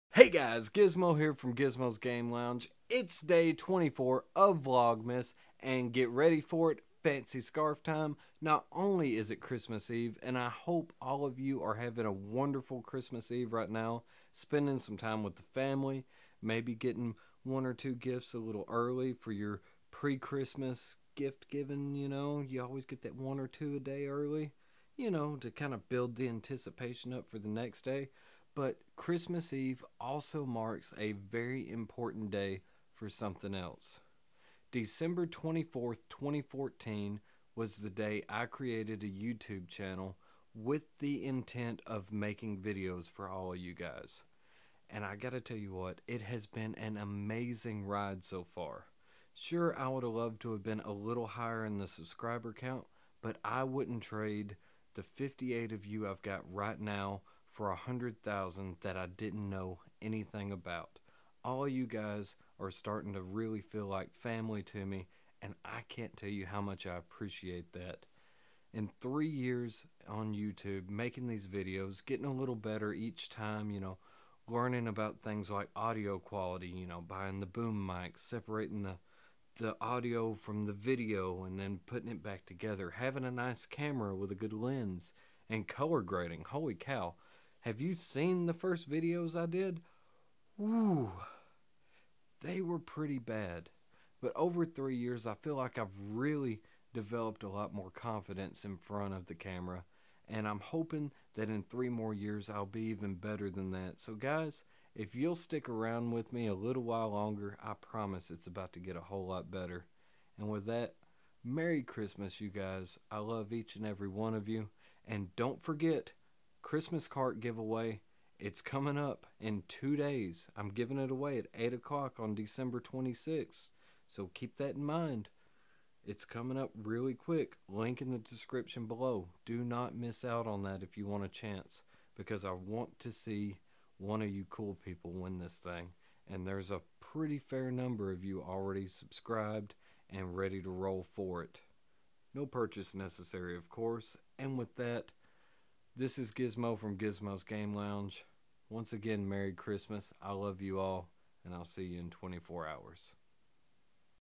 Vlogmas Day 24: A Very Special Day For Many Reasons
Today marks 3 years since I created a Youtube channel. It is crazy to think it has been 3 years and what a crazy ride it has been.